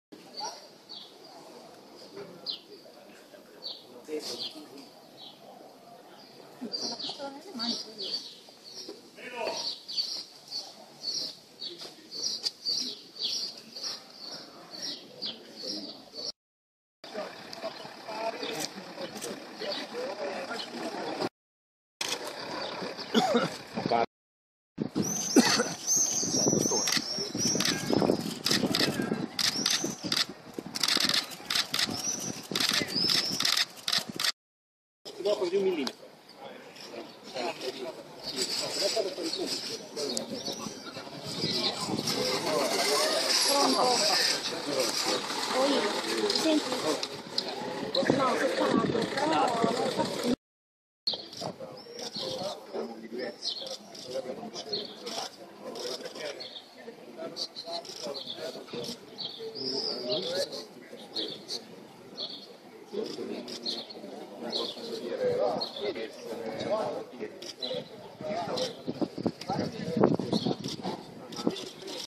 Cervia - omicida in fuga.wmv
Dopo l'omicidio compiuto a Cesena nella mattina del 31 maggio, il killer è asserragliato nel duomo di Cervia.